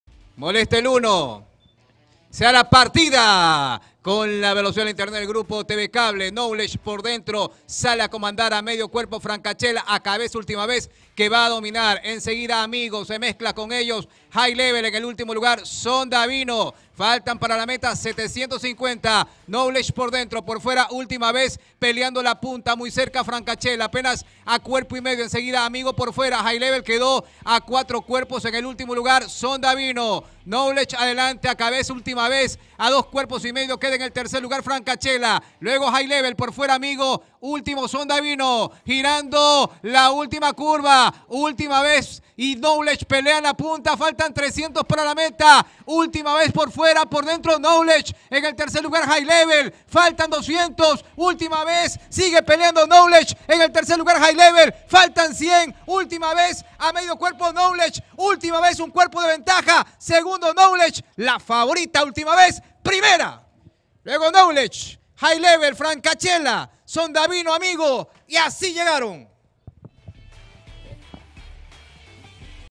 2020-211 Última Vez (Ecu) sept. 27, 2020 - Hipód. Miguel Salem Dibo - Revista La Fija
ÚLTIMA VEZ (Ecu-2016) hembra castaña por Sahara Heat y Último Beso (Domingo). Quinta victoria en 23 salidas. Jinete: Gustavo Luque (Per); Preparador: Jaime Alfredo Estada (Col); Stud: Eva María. Haras: Eva María. Distancia 1.000 metros. Tiempo: 1’00”1 (pista liviana).
1º) Última Vez (Ecu), 2º) Knowledge (Arg), 3º) High Level (Ven). No corrió Quique Azul (Arg).